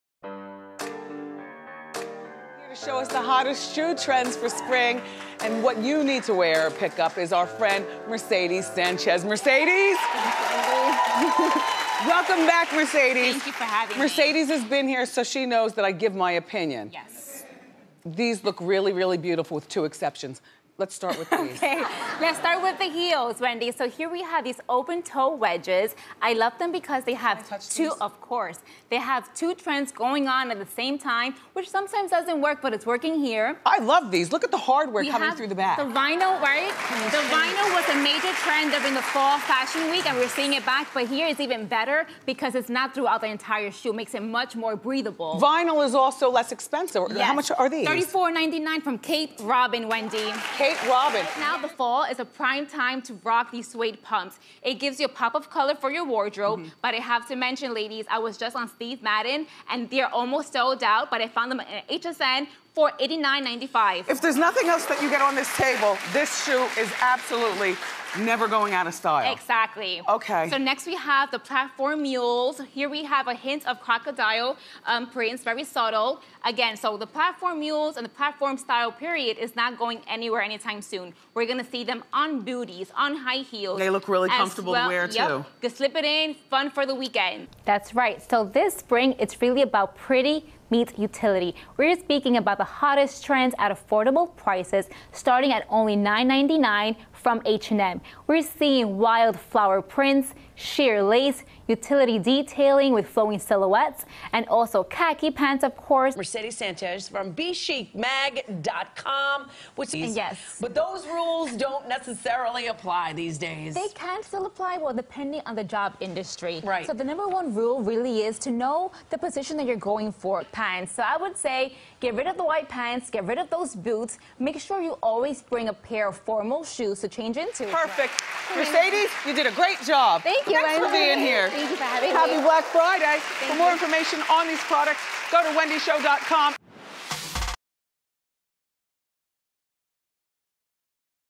Lifestyle Expert TV Reel Mercedes Sanchez
Lifestyle Expert Mercedes Sanchez shares clips from recent TV segments.